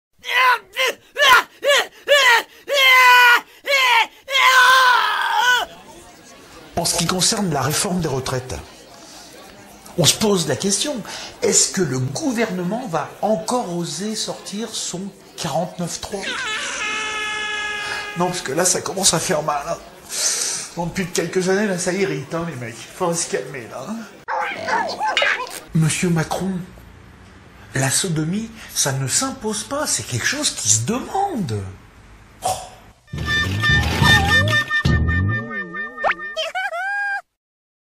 Au bout d'un moment...ca irrite :-(
Réforme des retraites. Le gouvernement va t-il encore nous ressortir son fameux 49.3 ?